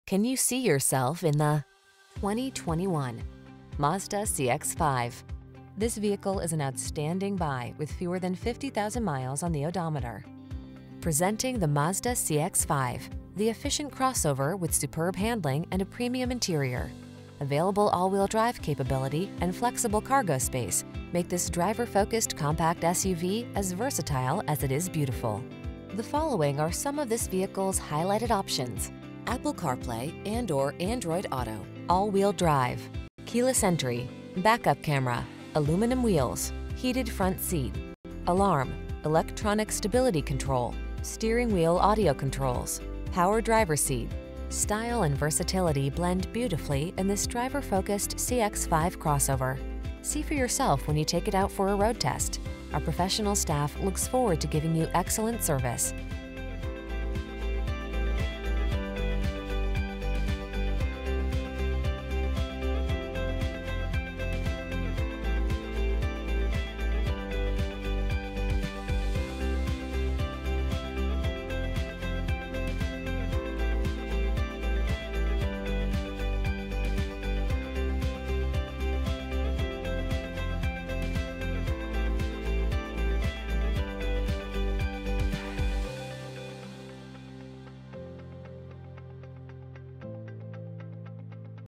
2021 Mazda CX-5 Touring CT Milford, Danbury, Stamford, New Haven, Hamden
Pre-Owned 2021 Mazda CX-5 Touring available at Mazda of Milford located in 915 Boston Post Road, Milford, CT, 06460. Servicing the Milford, Danbury, Stamford, New Haven, Hamden area.

Can you see yourself in the  2021  Mazda CX-5.  Presenting the Mazda CX-5, the efficient  crossover with superb handling and a premium interior. Available all-wheel-drive capability and flexible cargo space make this driver-focused compact SUV as versatile as it is beautiful. The following are some of this vehicles highlighted options: Apple Carplay®/Android Auto®, All Wheel Drive, Keyless Entry, Back-Up Camera, Aluminum Wheels, Heated Front Seat(s), Alarm, Electronic Stability Control, Steering Wheel Audio Controls, Power Driver Seat. Style and versatility blend beautifully in this driver-focused CX-5 crossover. See for yourself when you take it out for a road test. Our professional staff looks forward to giving you excellent service!